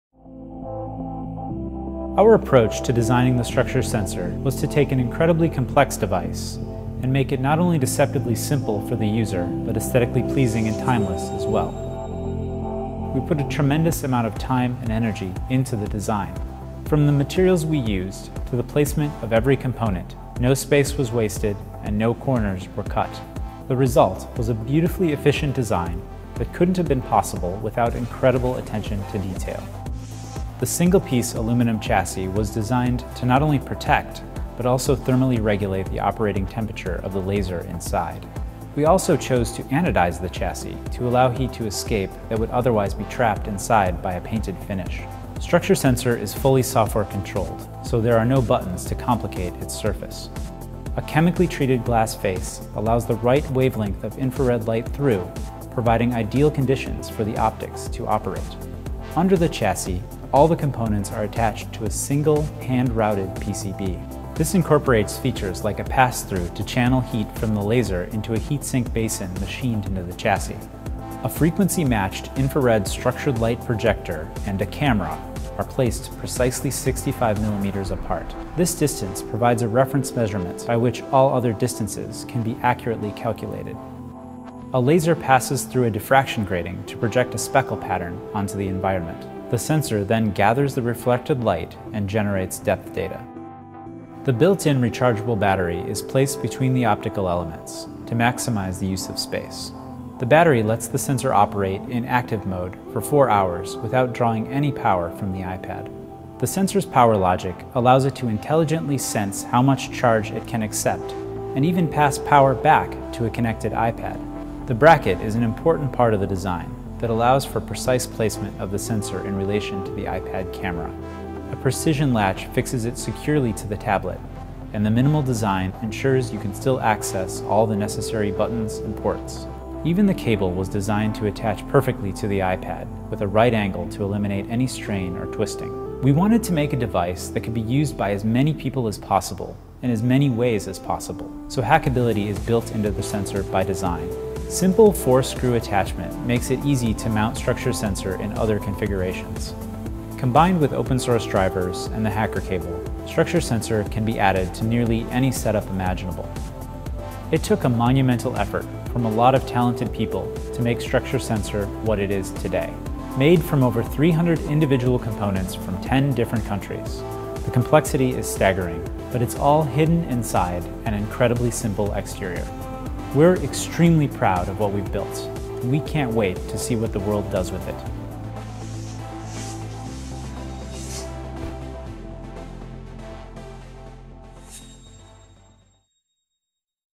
Occipital Structure Sensor Design Video
3D Scanning for iOS Devices - Occipital.

Easy to use. Hard to believe.

With the Structure Sensor, the future is in your hands. Rapid 3D scanning of objects and people, 3D maps of interior spaces, and mixed reality experiences where fantasy becomes impossible to tell from reality.

3D scanning, augmented reality, and more for mobile devices.
The Structure Sensor in depth.

Much of what makes the Structure Sensor revolutionary is hard to describe with just words. And, just as seeing the Structure Sensor in action reveals its captivating capabilities, so do the details and decisions that went into its design reveal its uncompromising quality.